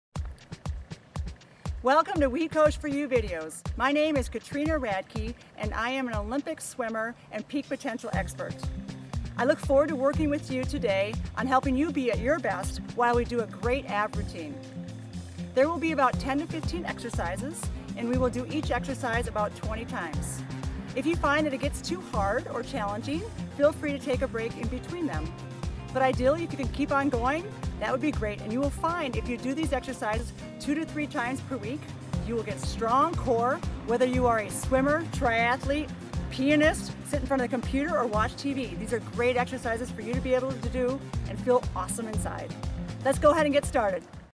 Katrina Radke Ab promo with music.mov
Olympic swimmer Katrina Radke introduces the ab routine video, for all people - athletes, corporate people, anyone! Take care of your body and mind. Exercise and feel good!
Videos available June 2012.